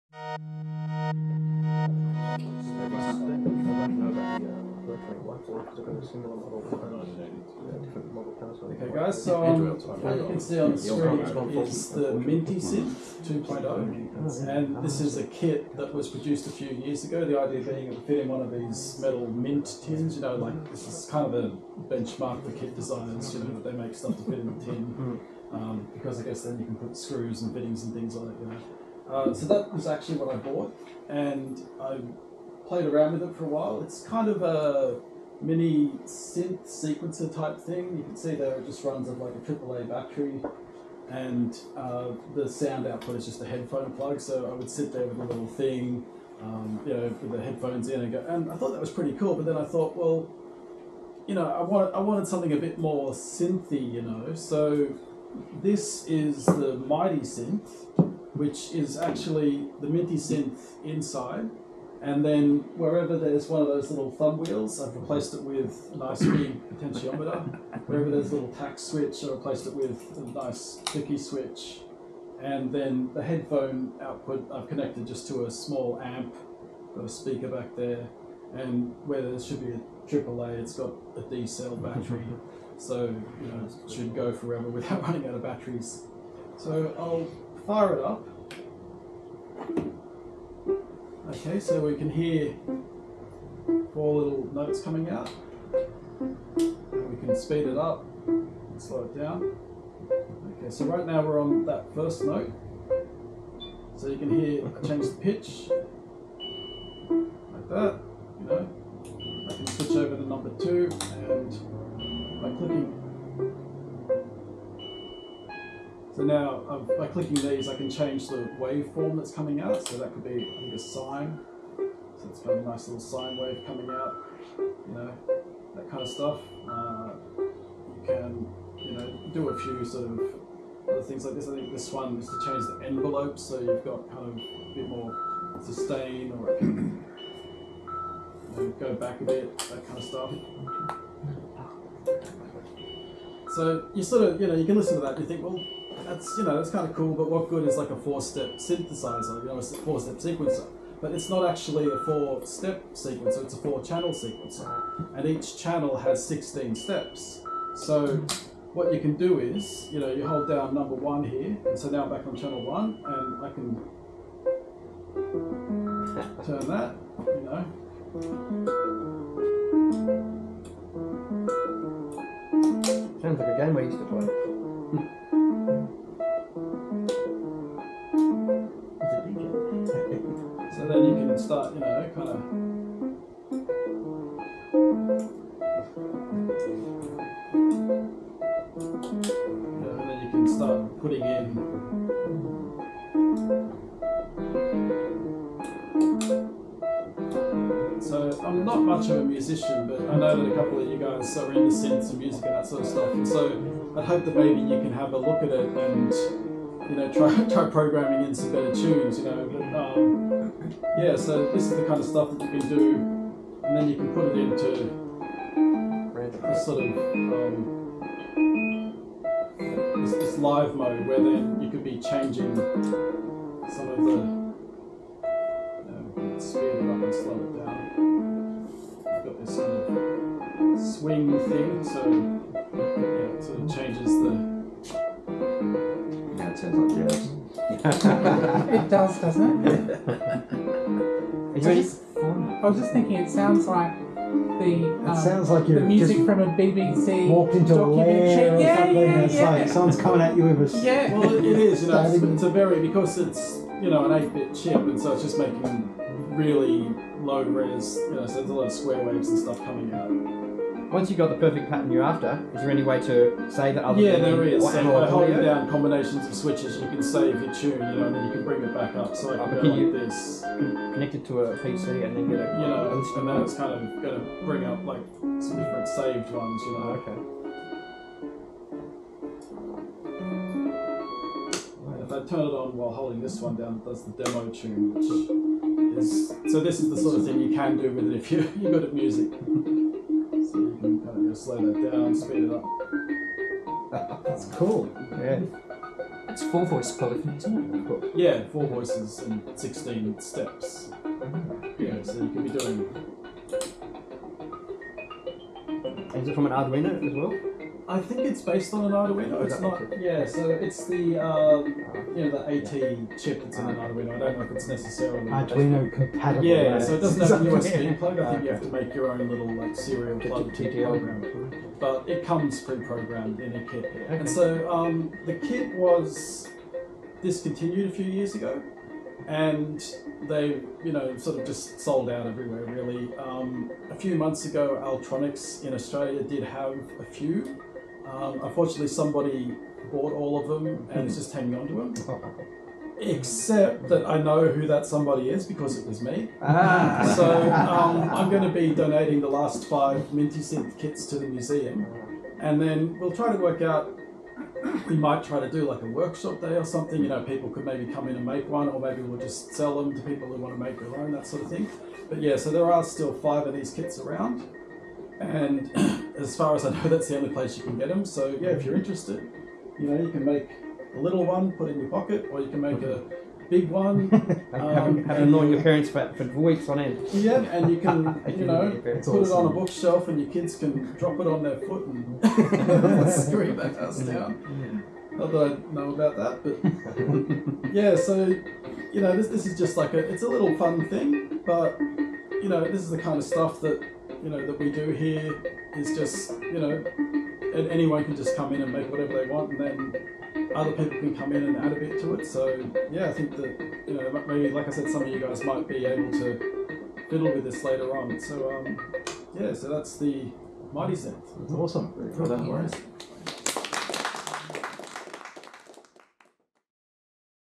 ACMS Presents - Adrian Magni with the MightySynth 2.0
The MightySynth is a super-sized version of the MintySynth kit, putting a surprisingly full-functioned synth and sequencer in the palm of your hand.

This kit has been discontinued by the manufacturer, but the ACMS still has a few in stock if you would like to make your own.